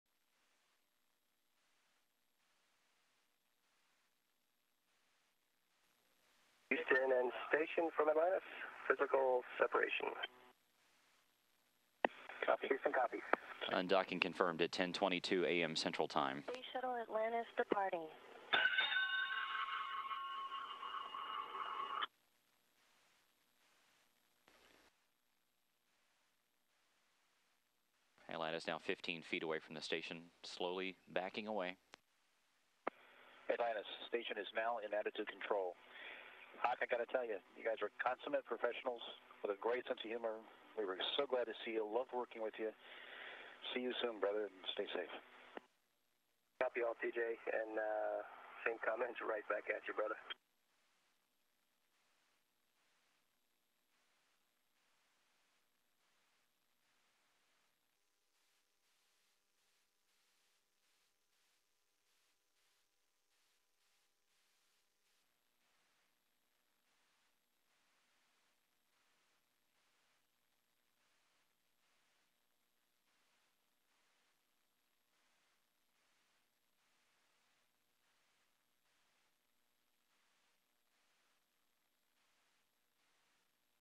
Shuttle Atlantis undocks from space station
1. Space shuttle Atlantis orbiting in space
2. Various of the two space craft undocking
3. UPSOUND (English) Atlantis crew talking to Mission Control:
"Atlantis: Houston and station from Atlantis, physical separation,"
UPSOUND (English) Houston Mission Control: "Copy, Houston copy,"
UPSOUND (English) Houston Mission Control: "Undocking confirmed at 10:22am Central Time,"
UPSOUND (English) Atlantis: "Space Shuttle Atlantis departing."
4. Close of Atlantis pulling away from space station
5. Close of Atlantis pulling away from space station UPSOUND (English) Houston Mission Control: "Atlantis now 15 feet away from the station, slowly backing away."
6. Close shot of Atlantis pulling away from space station UPSOUND (English) Space Station flight engineer T.J. Creamer talking to Atlantis Commander Ken Ham: "Ham, I got to tell you, you guys are consummate professionals with a great sense of humour.  We were so glad to see you, loved working with you, see you soon brother, stay safe,"
UPSOUND (English) Atlantis Commander Ken Ham: "Copy all, T.J. and same comments right back at you, brother."
7. International Space Station orbiting in space
8. Atlantis moving away from the space station
9. Pan from International Space Station to the shuttle, orbiting separately
STORYLINE:
After a week of flying together, the space shuttle Atlantis undocked from a larger and virtually completed International Space Station on Sunday and headed for home on its final voyage.
The two orbiting craft parted company 220 miles (354 kilometres) above the Indian Ocean.
Once more, the space station astronauts thanked their shuttle friends for all their help and urged them to "stay safe."
Atlantis isn't set to land until Wednesday.
On Monday, the six crewmen will conduct one last safety inspection of their ship.
Before they separated, two commanding officers shook hands twice and laughed, then embraced.
Their crewmates - representing the United States, Russia and Japan - followed suit.
Earlier in the morning, Ham told reporters the two crews had fun.
He said all 12 space fliers bonded through hard work and performed as a single team.
The space station is bigger and packs more power, thanks to Atlantis and its crewmen.
They left behind a new Russian compartment packed with supplies, as well as six fresh batteries and other equipment that was hooked up during a series of spacewalks.
Its total mass exceeds 816-thousand pounds (370-thousand kilograms), and it's 98 percent complete in terms of living space.
Two shuttle missions remain to wrap up NASA's share of construction.
Discovery is scheduled to fly in September, followed by Endeavour in November.
As it currently stands, once Atlantis lands, it will never fly in space again.
NASA and some politicians are pushing hard for another mission, however, so Atlantis can haul up a final load of supplies in June 2011.
The White House - which wants NASA concentrated on getting astronauts to asteroids and Mars in the coming decades - would need to approve any extra flights.